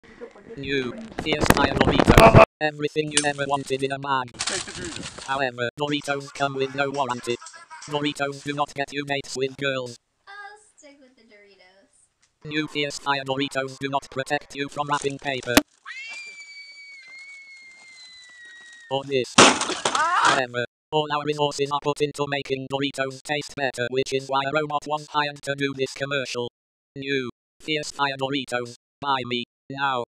Fierce Fire Doritos - Fierce Fire Doritos
Frito Lay Company has engineered a new flavour. All available resources were aimed at the taste of the doritos leaving a low budget commercial.

New Fierce Fire Doritos. Buy me. Now.